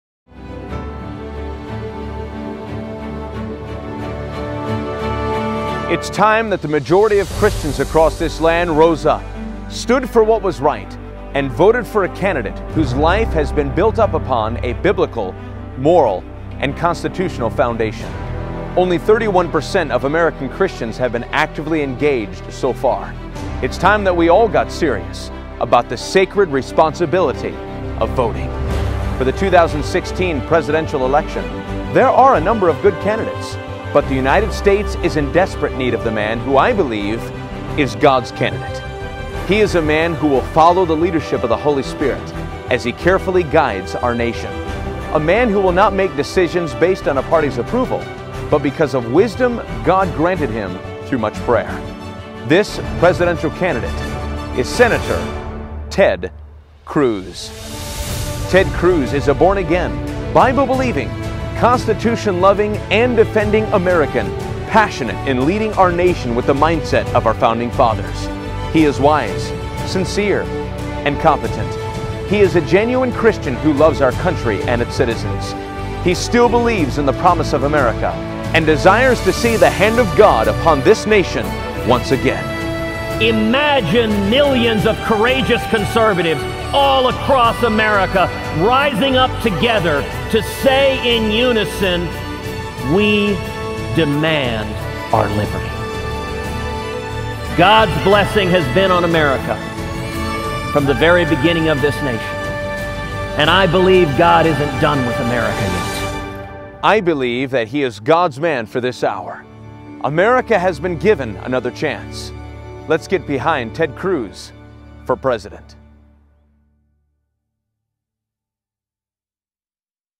Ted Cruz for U.S. President
It's time that God's people rose up as a generation of courageous conservatives and reignited the promise of America. God has given our country another chance. Let's get behind Ted Cruz for President! Visit tedcruz.org and pledge your support today.